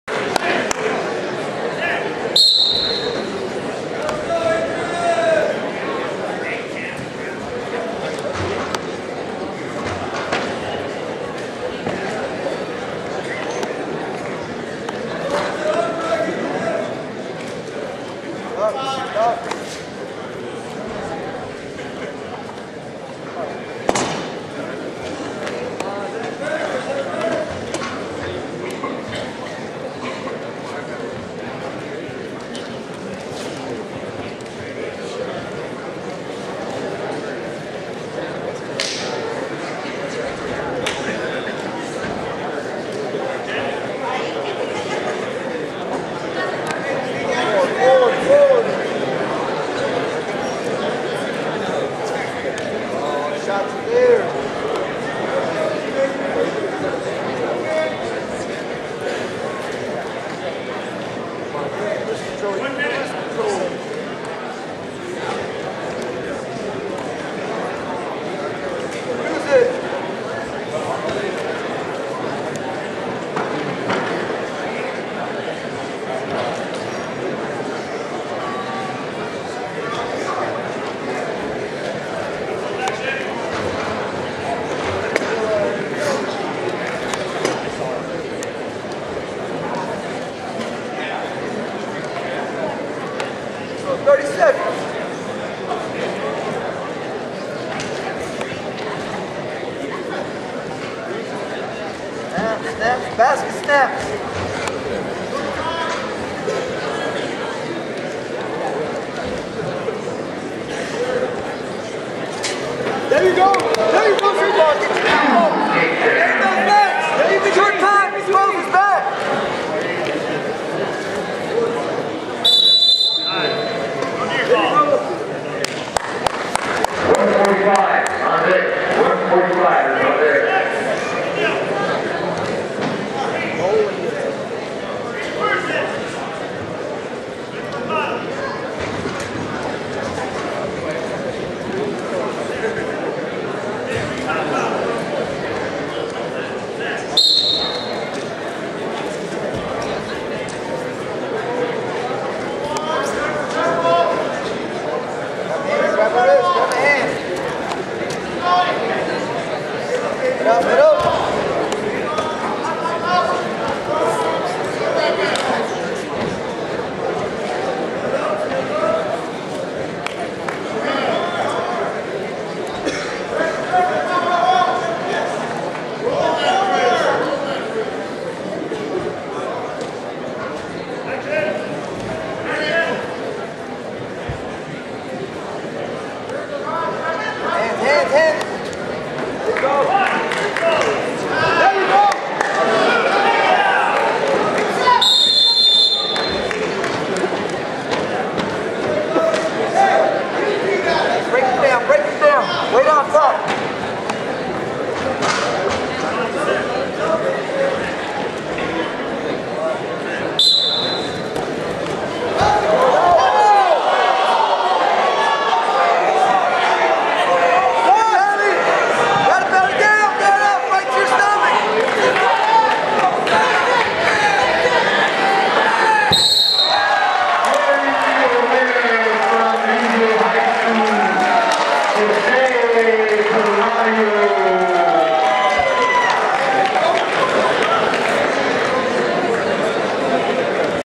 2018 Canyon Springs - Raul Huerta Invitational 220lbs
Jose Pelayo (Indio) vs. Adrian Guerra (Downey)